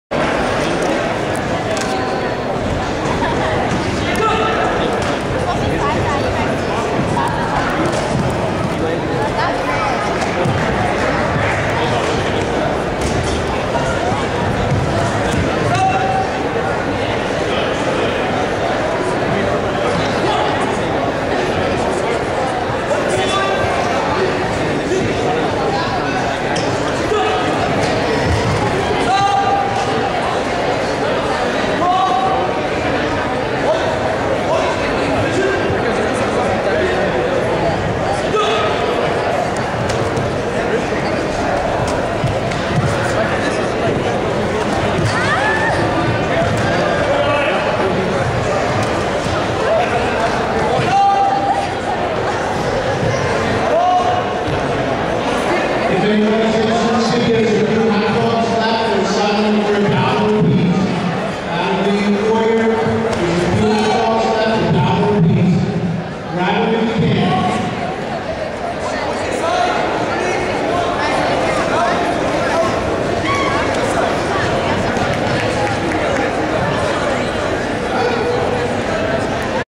USNA Karate - Mike Cristiano - Second fight
Mar 27, 2010 - United States Naval Academy attends Karate Competition in Thorofare, NJ.